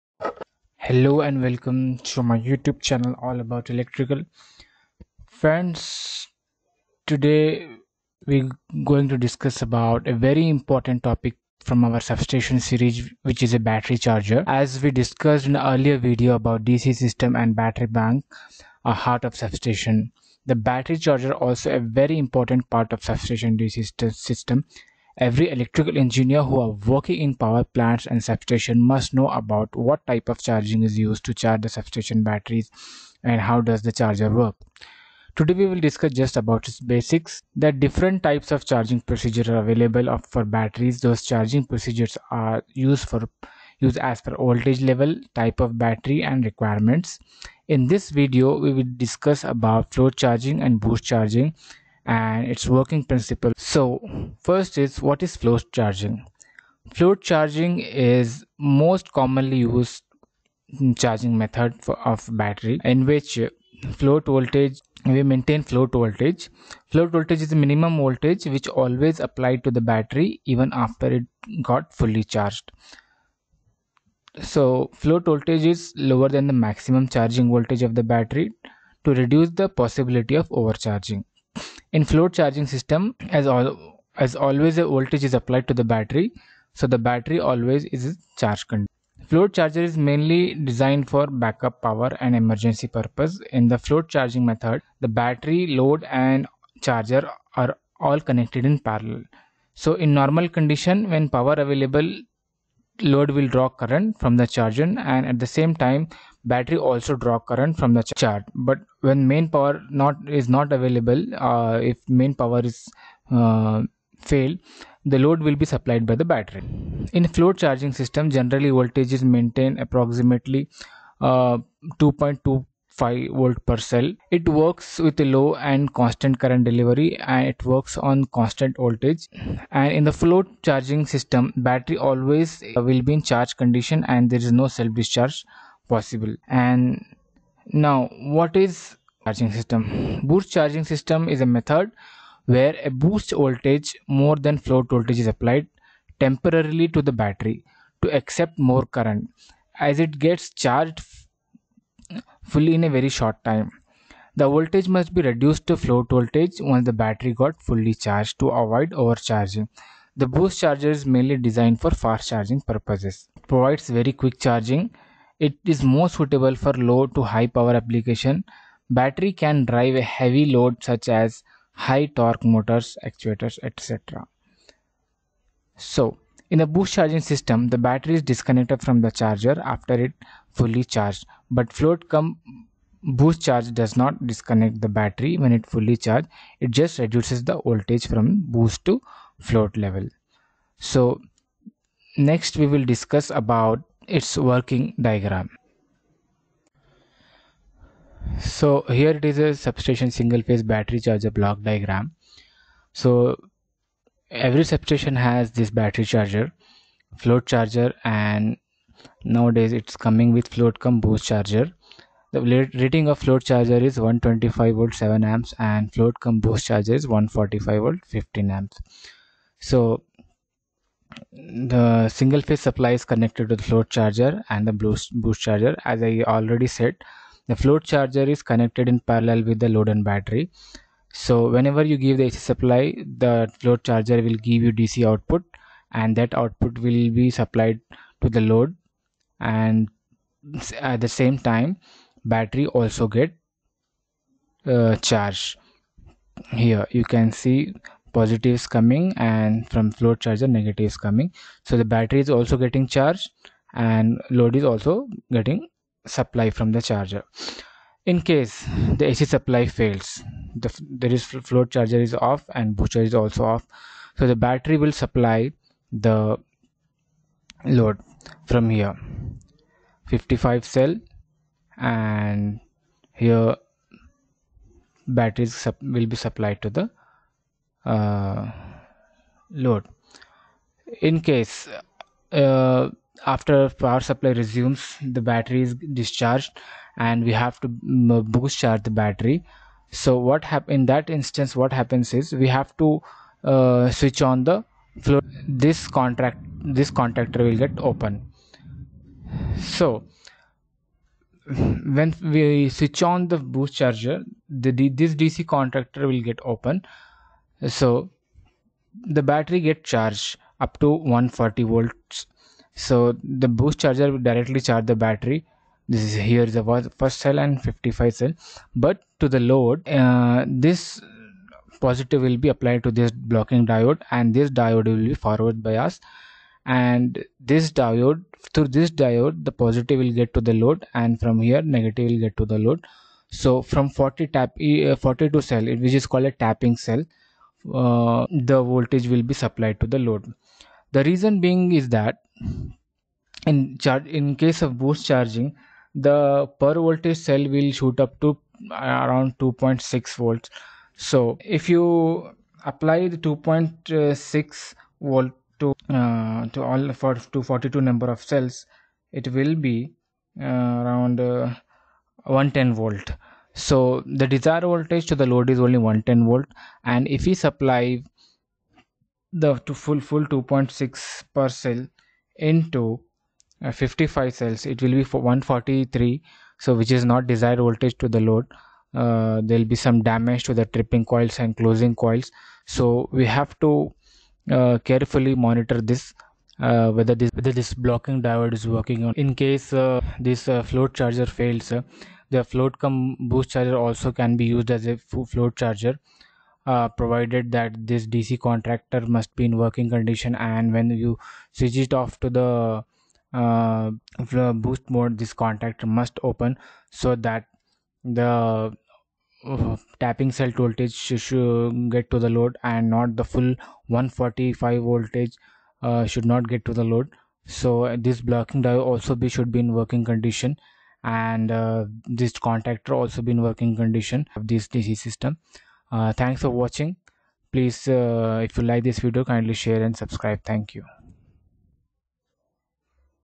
1phase110V sub station float and Float Cum Boost battery charger
this Video is about Sub-Station battery charger ,its importance what is float and boost charging and Charger working principle,